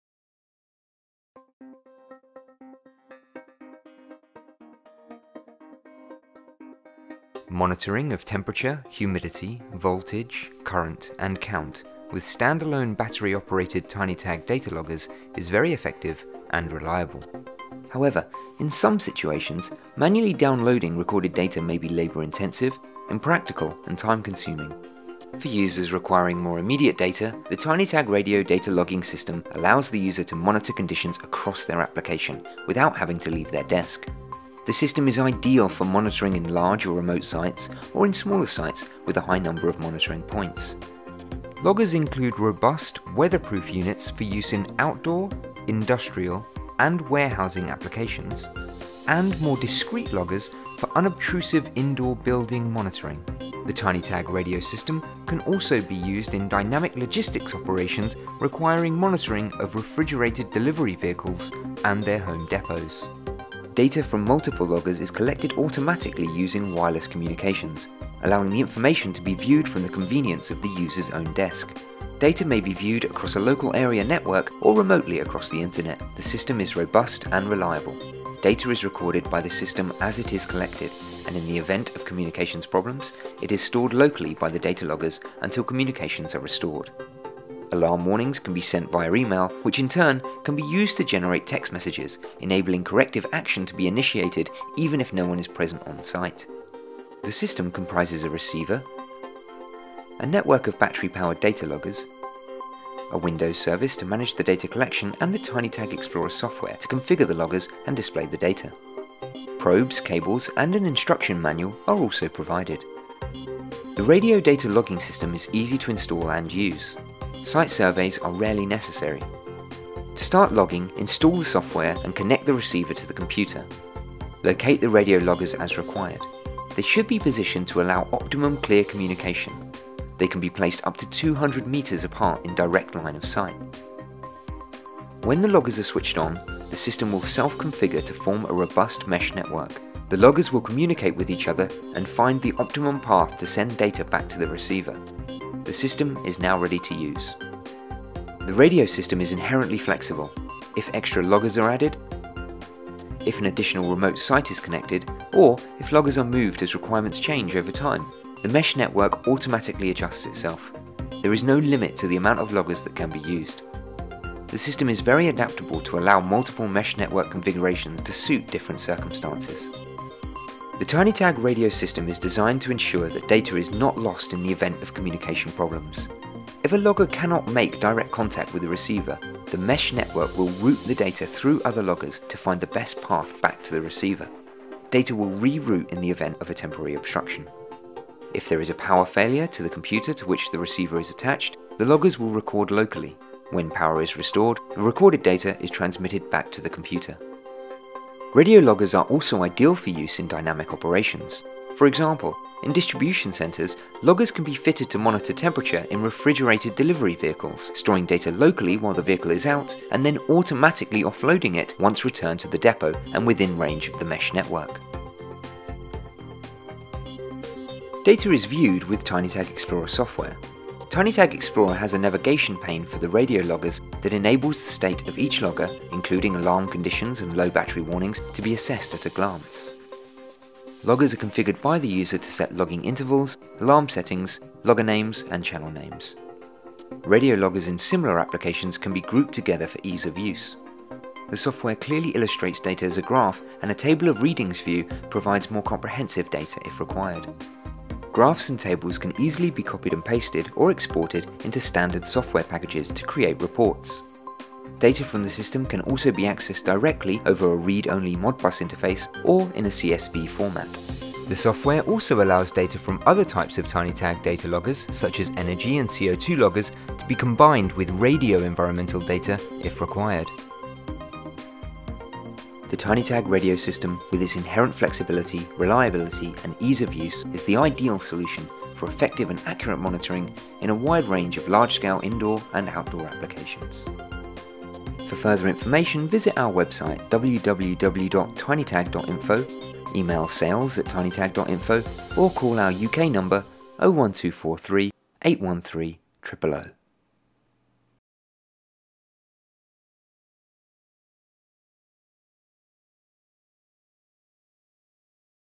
Tinytag Radio Data Logging System - 15.01.15
This video gives you an overview of the Tinytag Radio Data Logging System. It presents a straightforward guide to how the system works and its benefits. The Tinytag Radio Data Logging System monitors temperature and relative humidity of a wide range of applications (ideally larger site) from the convenience of the user's own desk. The system is easy to install and use, using wireless communications to enable data to be viewed and the loggers reconfigured without the need to collect and download them manually.